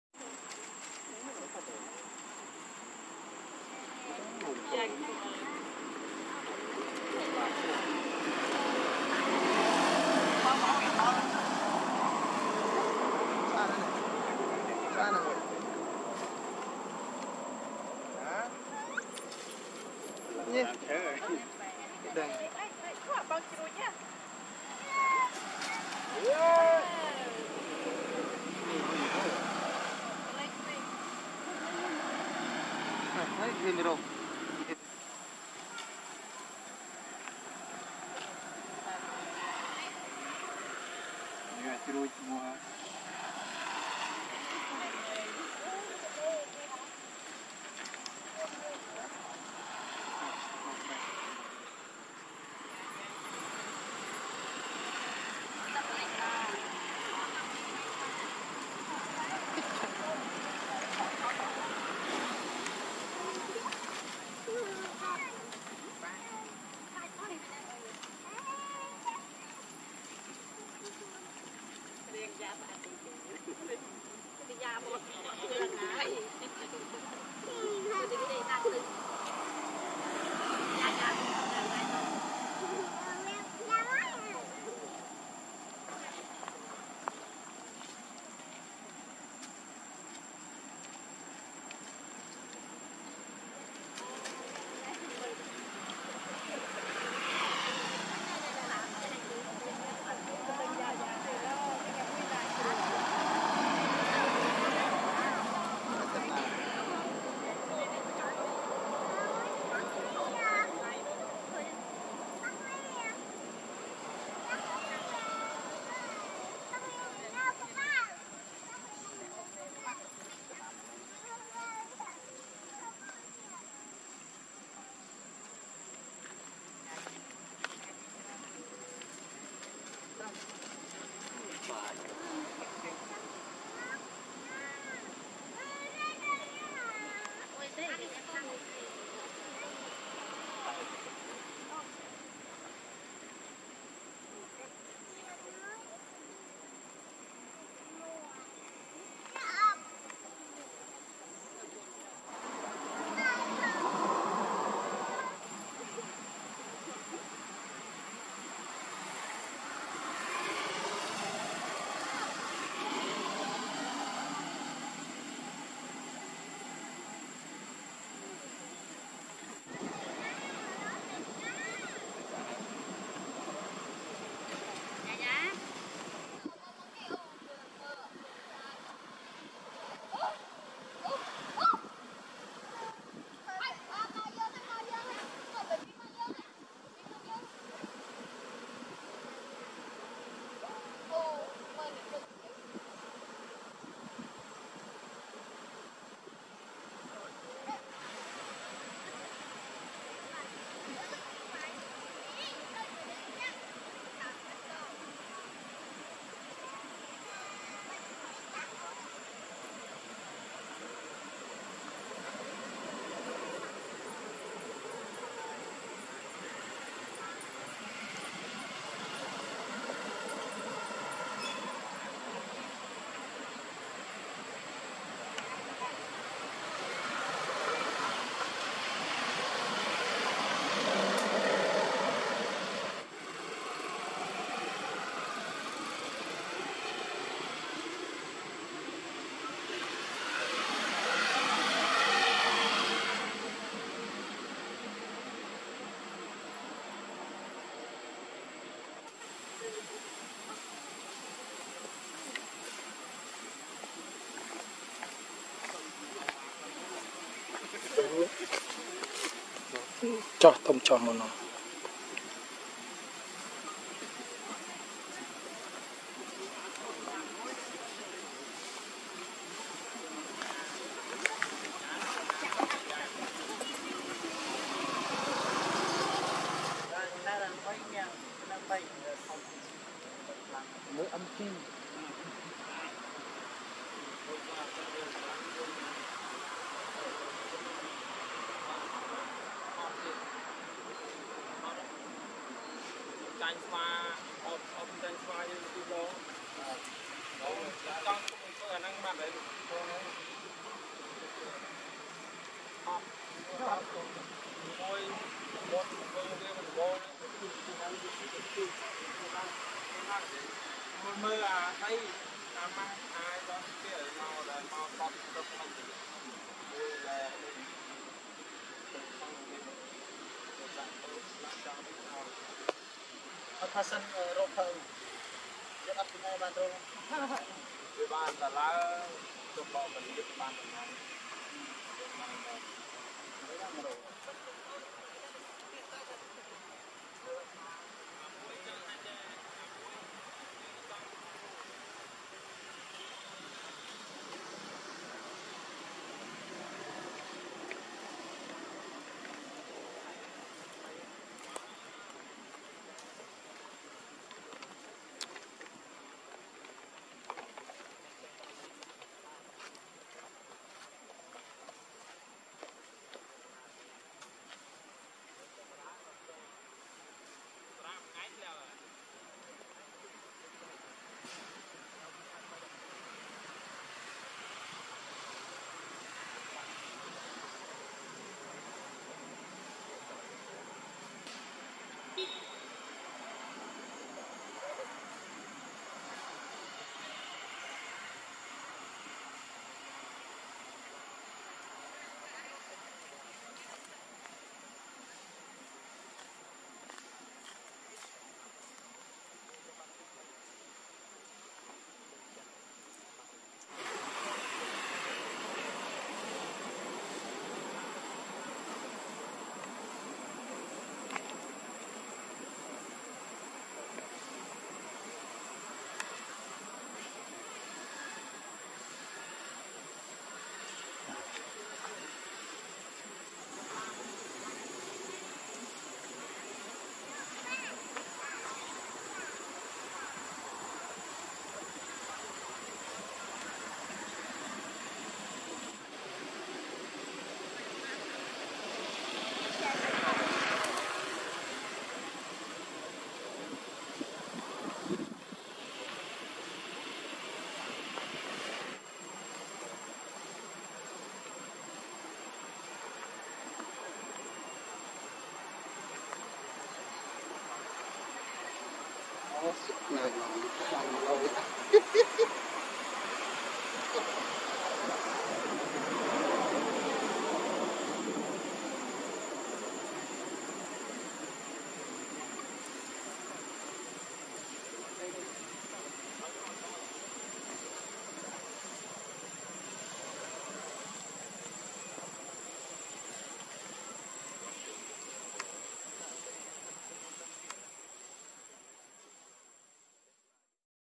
Today Baby Monkey YaYa Come And Visiting Young Mom Monkey Rose, Baby Rex And Amber Troop So Nice...
0:00 | Today Baby Monkey YaYa Come And Visiting Young Mom Monkey Rose, Baby Rex And Amber Troop So Nice...

Thanks!